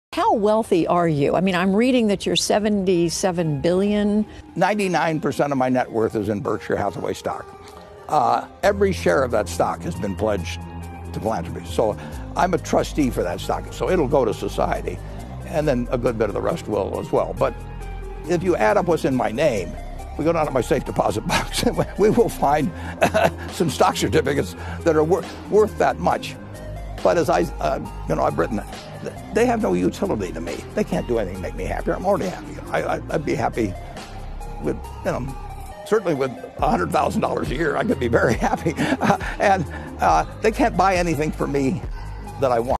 Richest 💰💰Warren buffet Tamil💰💰
Billionaire investor Warren Buffet says that earning an annual salary of $100,000 would make him happy. He explains that he doesn't need a lot of money to live a comfortable life, and that he would be just as happy with a modest income.

Watch the video to see why Warren Buffet says $100,000 a year is all he needs to be happy!